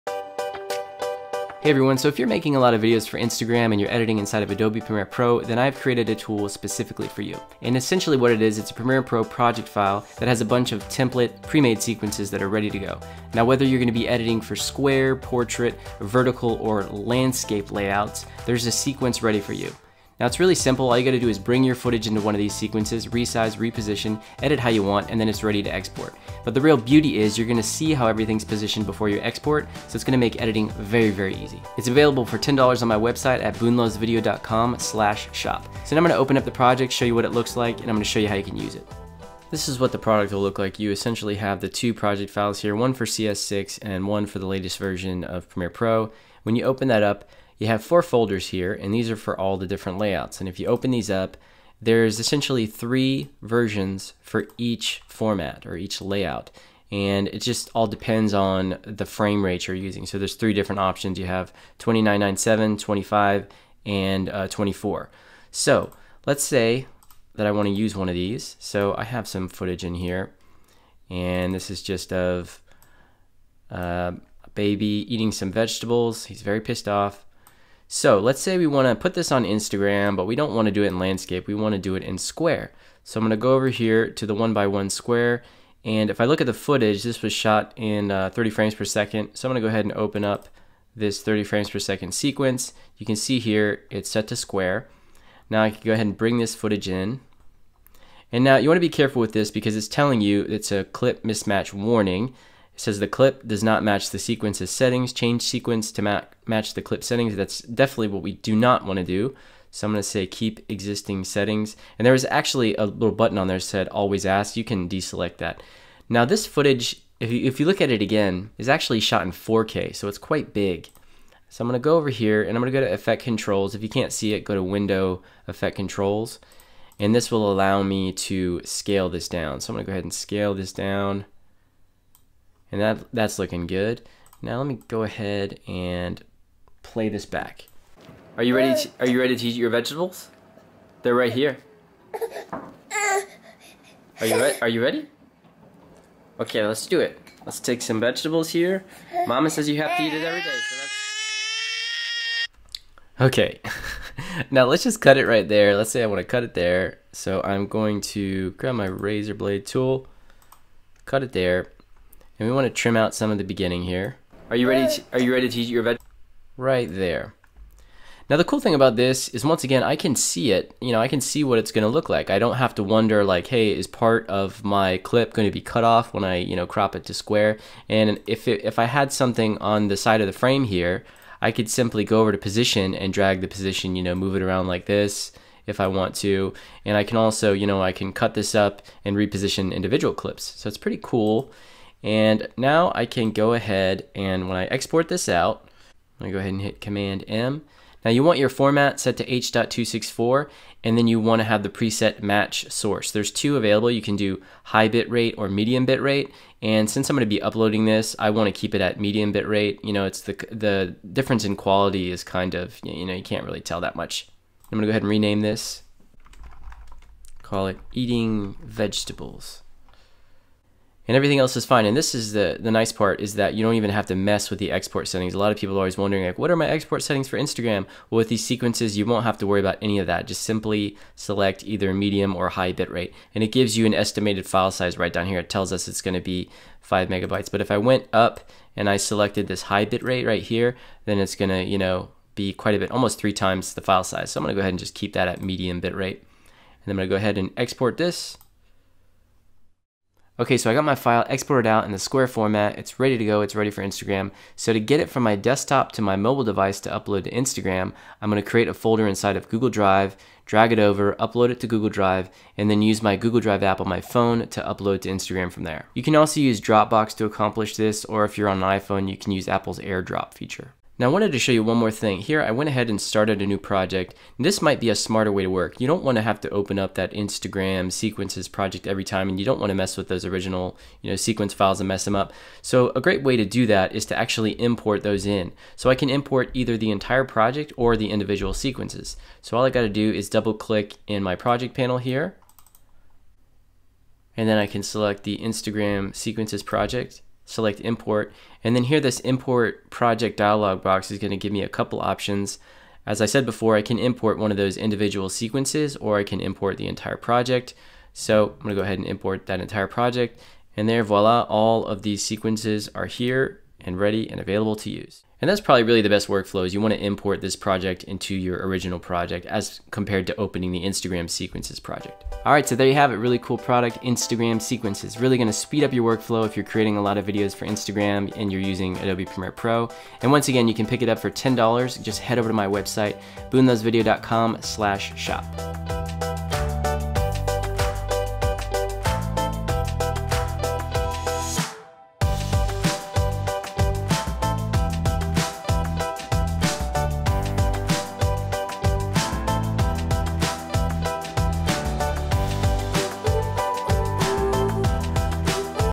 📱INSTAGRAM SEQUENCES for Premiere Pro | Product Tutorial
Quickly and easily edit video for Instagram inside of Adobe Premiere Pro with my Instagram Sequences Premiere Pro project file. Easy to use, the project comes with 12 different sequences, including Landscape, Vertical, Square, and Portrait resolutions. Available for Adobe Premiere Pro CS6 and all later versions. Stop wasting time on the technical side and focus on your content.